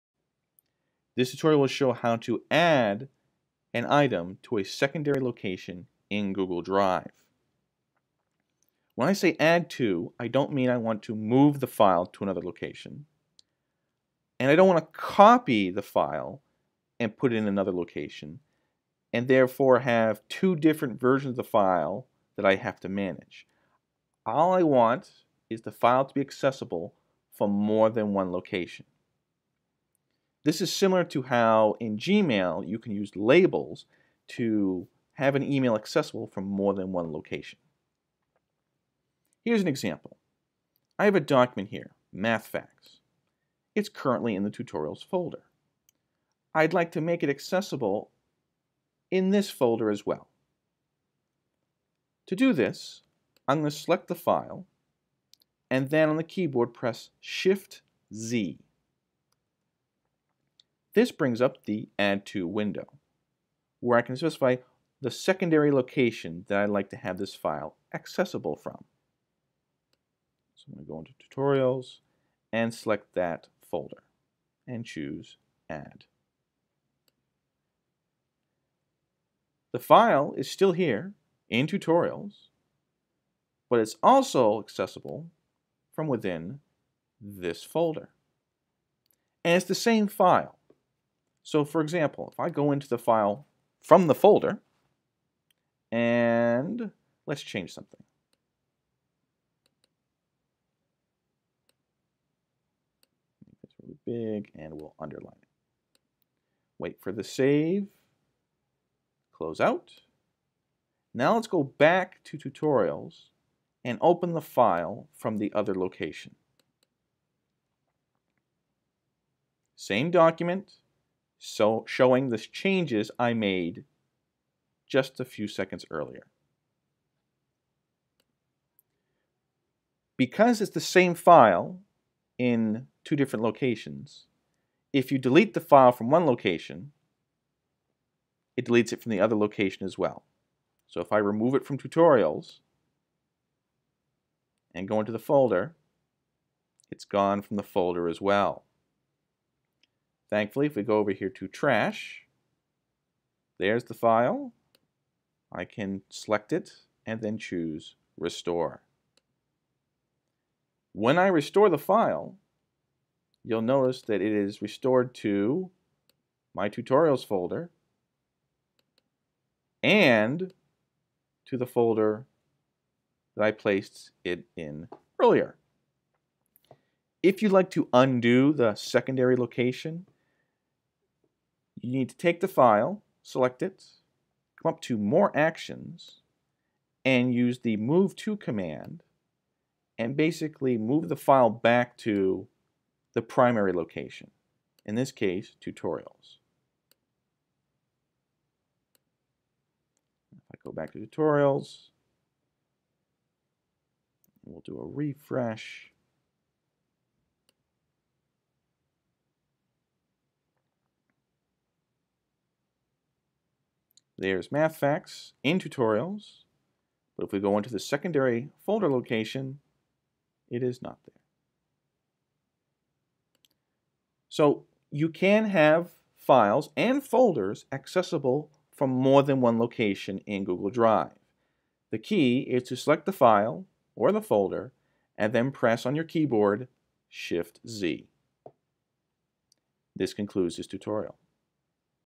HowTo - 'Add To' Secondary Location in Drive
This tutorial will show how to add an item in Google Drive to a secondary location. This is not the same as "moving" an item or "copying" an item. The "Add To" option allows you to create more than one path to access the file/folder.

This is similar to how labels work in the Gmail app.